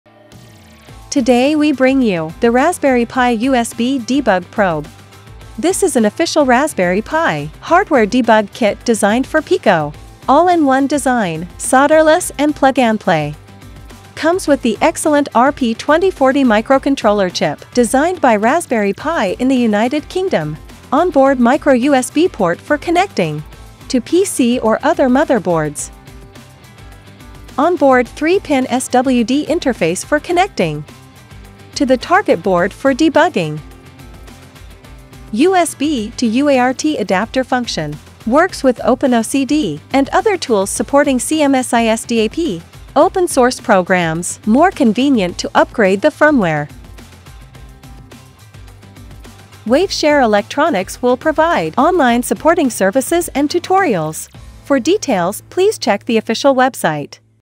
Raspberry Pi Original USB Debug Probe, Hardware Debug Kit Designed For Pico, Based On RP2040
Raspberry Pi Debug Probe is an official USB hardware debugger designed for Pico, all-in-one design, with the features of solderless and plug-and-play, can be connected to the debug interface of the target board via SWD interface.